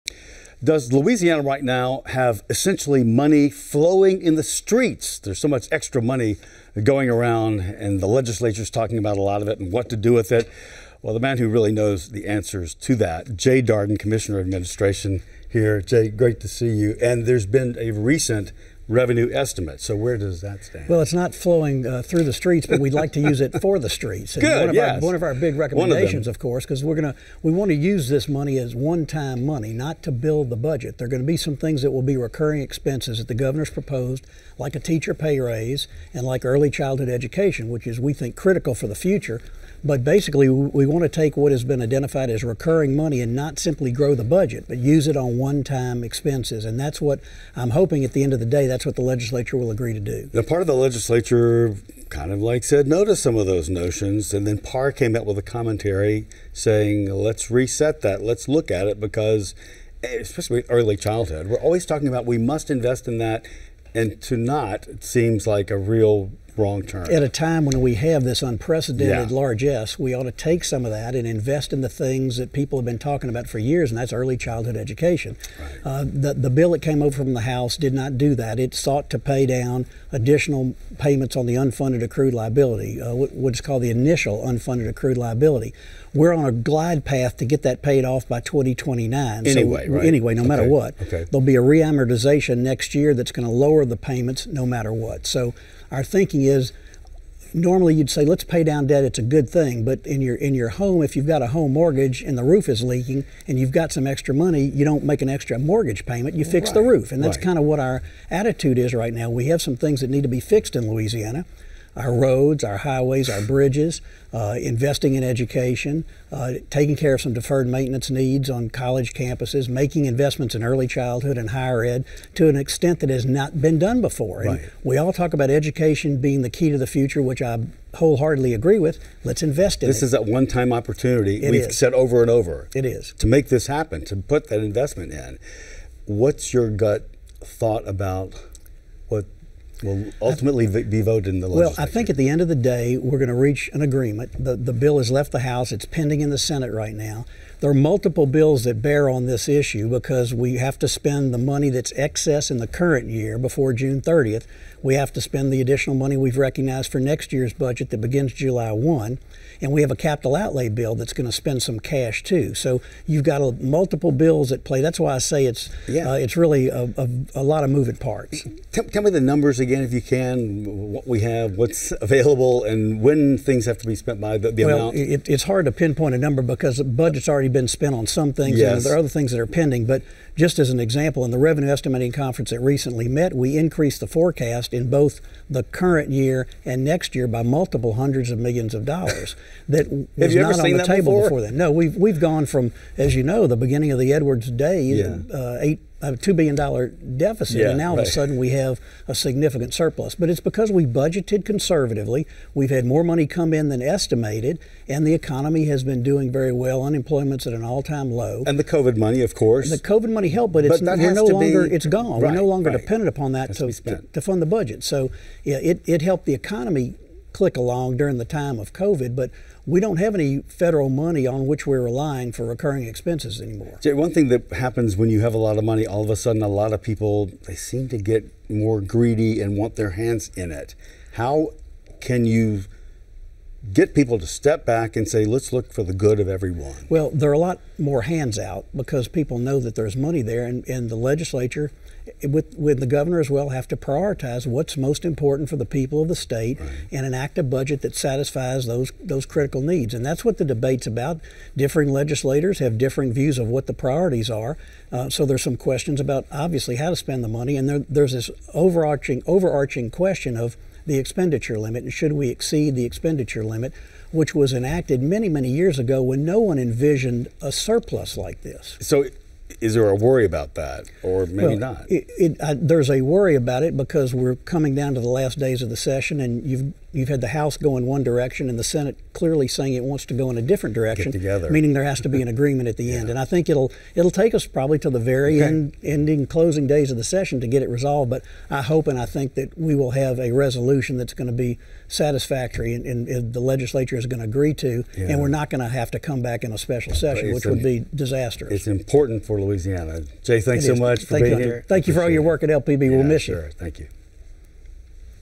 Big Budget Surplus | LSWI Segments | 05/26/2023
Louisiana is looking at an historic budget surplus - $1.9 billion. Now lawmakers are torn on what to do with the cash: invest in infrastructure or pay down debt.